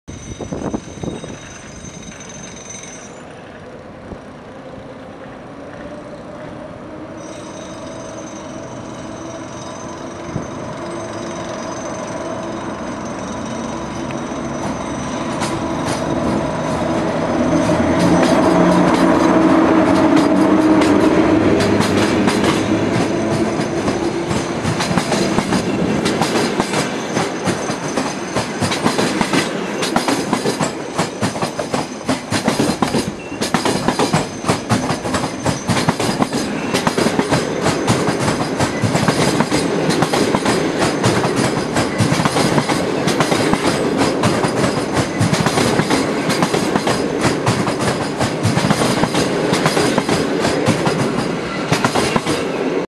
10/1011 '' Polonez '' Warszawa Zachodnia - Moskwa / Terespol
EU07-421 + '' Polonez '' do Moskwy odjeżdża z Warszawy Wschodniej. 3 ostatnie wagony jechały do Terespolu.